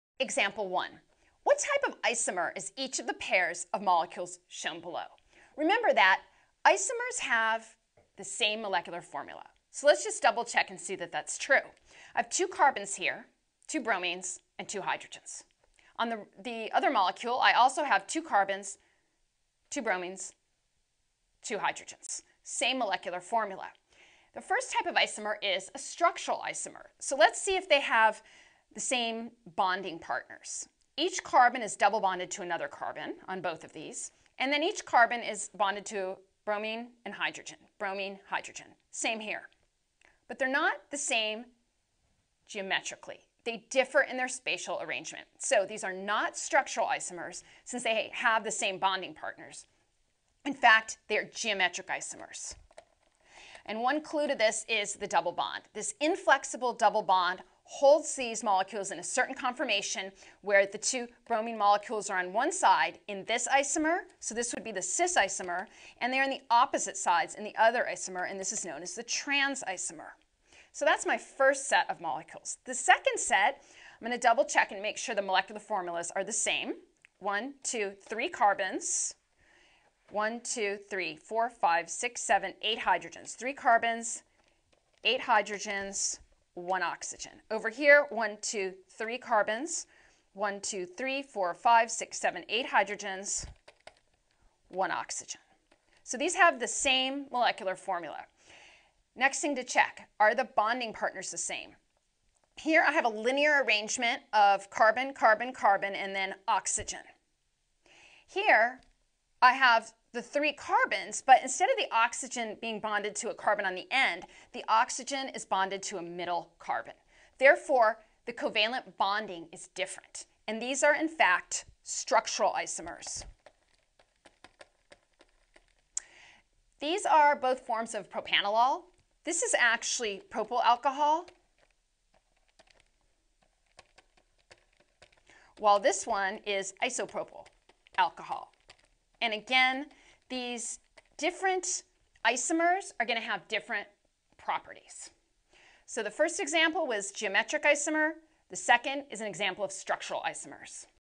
Type of Isomer
Full example on types of isomers from Educator.com’s AP Biology class. Want to know more? Our full lesson includes in-depth video explanations of organic compounds.

In this video, we’ll go over an example to identify the type of isomers found in molecules. She also goes over organic compounds, inorganic compounds, isomers, structural isomers, geometric isomers, and functional groups. This will be useful in AP Biology when determining the isomer in any molecule compounds.